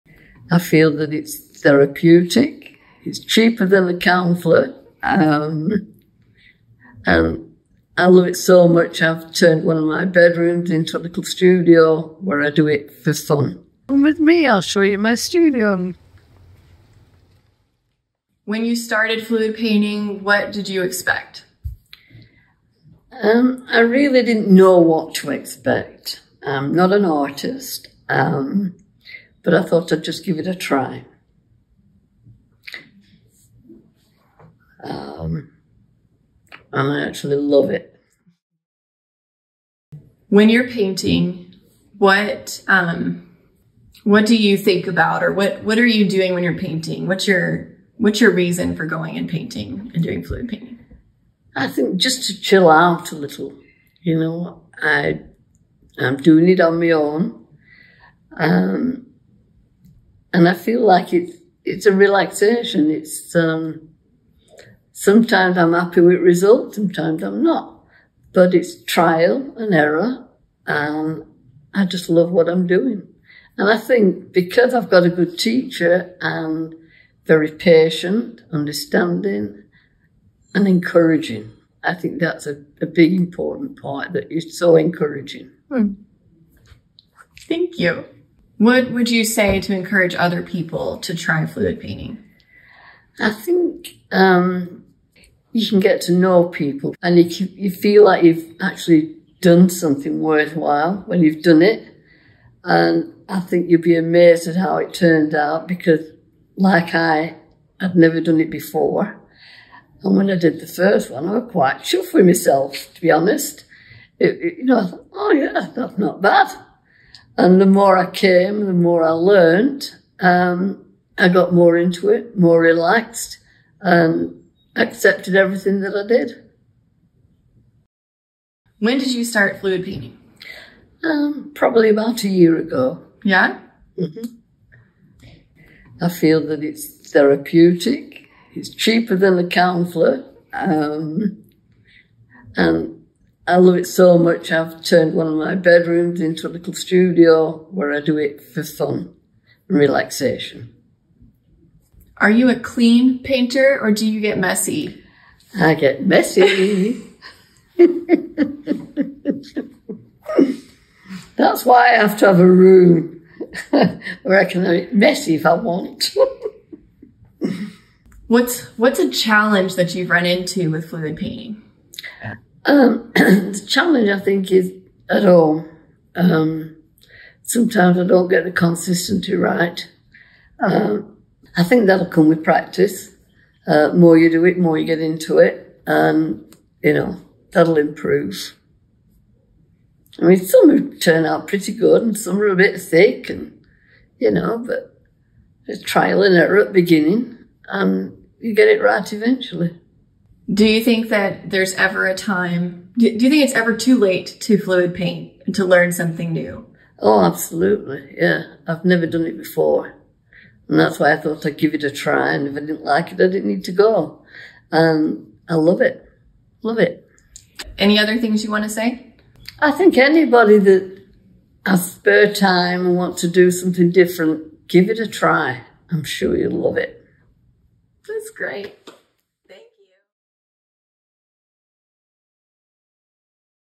It's cheaper than therapy! (Marlene's story)
Marlene started fluid painting with me last year. She was apprehensive to start as she had had a poor experience with a previous art teacher who was critical of her art. After we talked on the phone, and I promised her that I'm never harsh, she agreed to come give fluid painting a try!

After the first painting she created, Marlene was hooked! She's a repeat visitor of my classes where she's developed her technique. Now she paints as a form of therapy and has so much fun as she experiments and plays with the paints.

She now has her own home studio, full of beautiful paintings, where she continues to create amazing work. Recently, Marlene and her husband took 8 paintings all the way to England to gift to her family!

Marlene’s story shows that it’s never too late to start fluid painting and having fun with creativity!